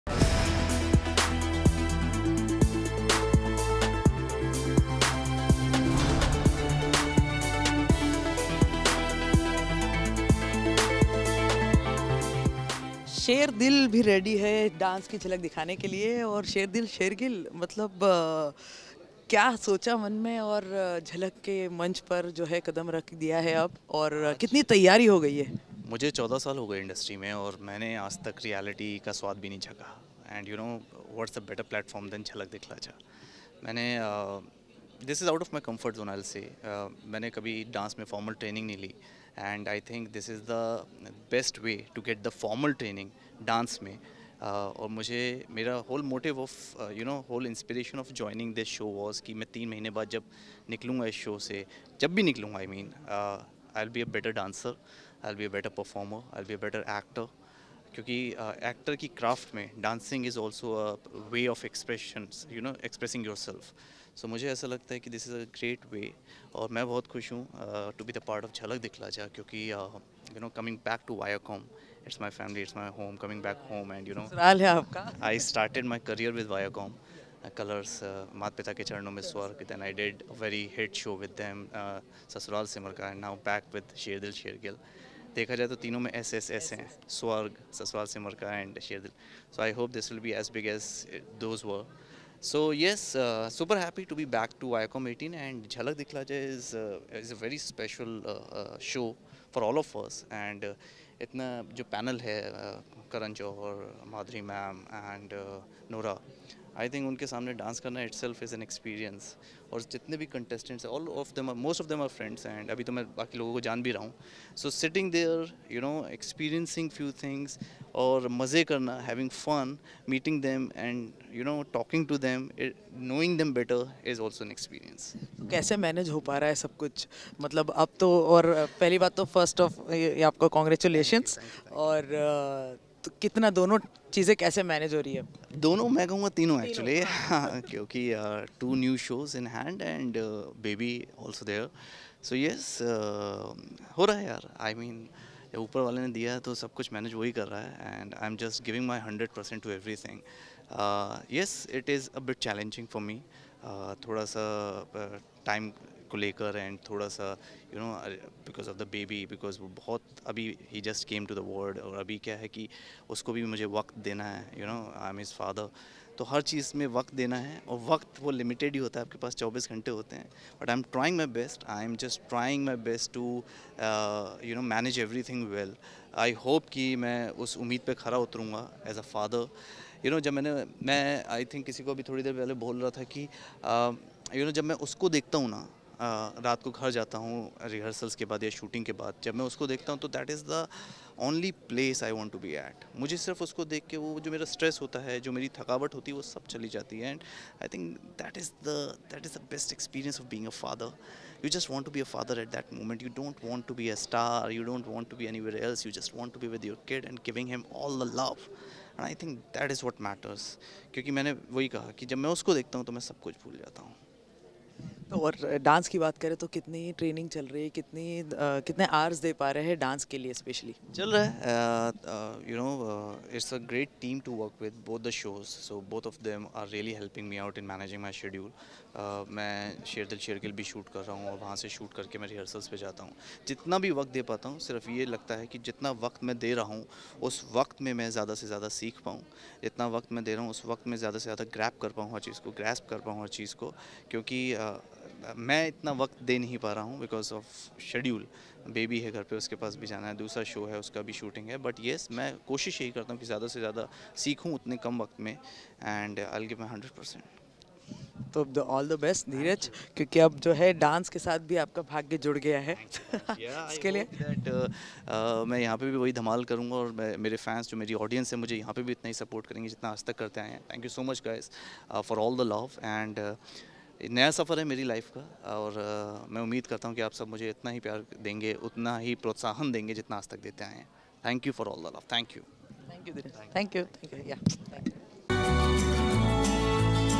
Dheeraj Dhoopar Excited For His Reality DEBUT in Jhalak Dhikla Jaa 10
Check out Dheeraj Dhoopar talking about his Excitement for the Show